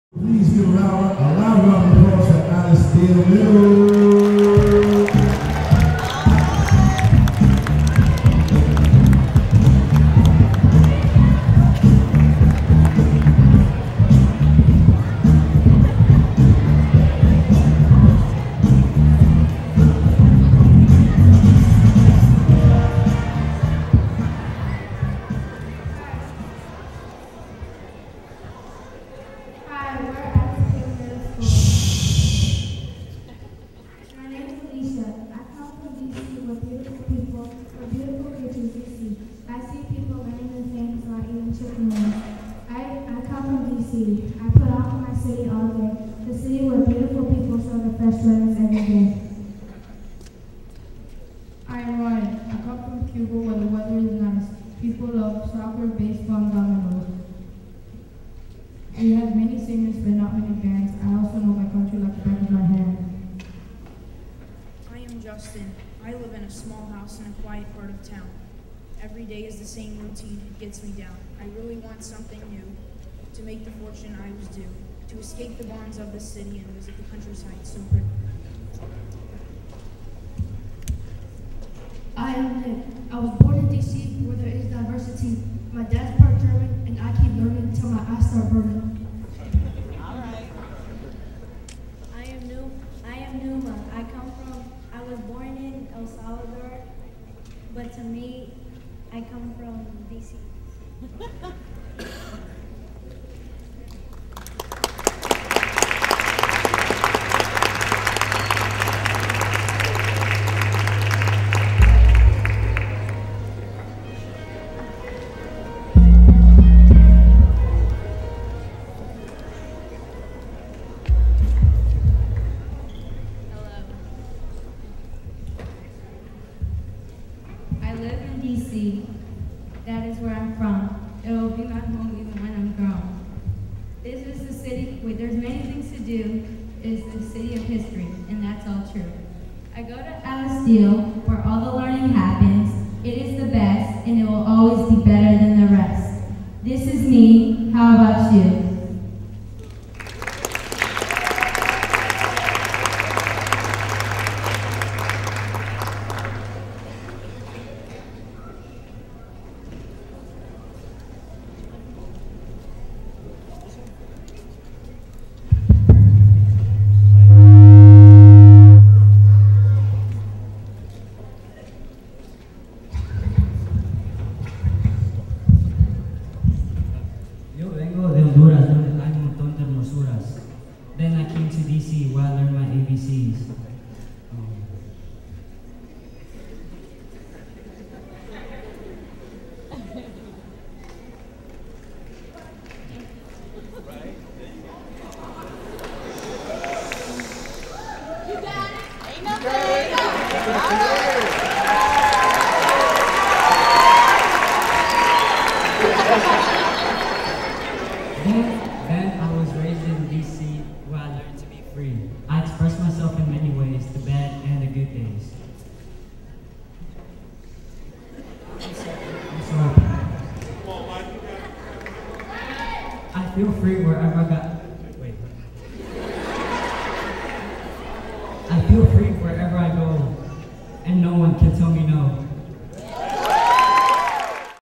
2013 DC SCORES Poetry Slam!: Alice Deal Middle School
Alice Deal Middle School performs at the 16th Annual DC SCORES Poetry Slam! on Dec. 4, 2013.